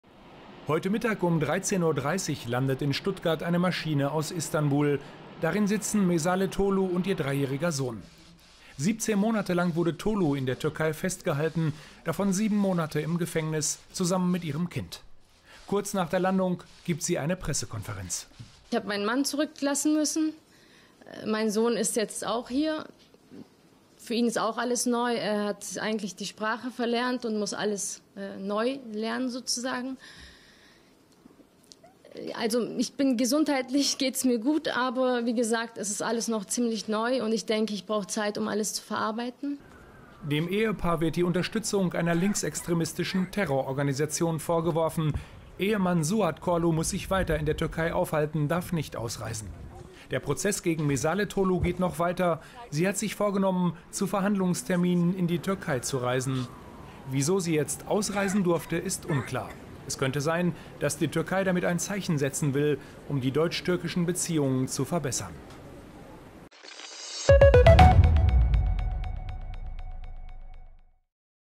Mesale Tolu zurück in Deutschland | BR24
Mesale Tolu ist wieder in Deutschland. Die Übersetzerin und Journalistin aus Neu-Ulm war über ein Jahr in der Türkei festgehalten worden. Sieben Monate davon saß sie im Gefängnis - wegen Terrorvorwürfen. Der Fall war auch eine schwere Belastung für die deutsch-türkischen Beziehungen.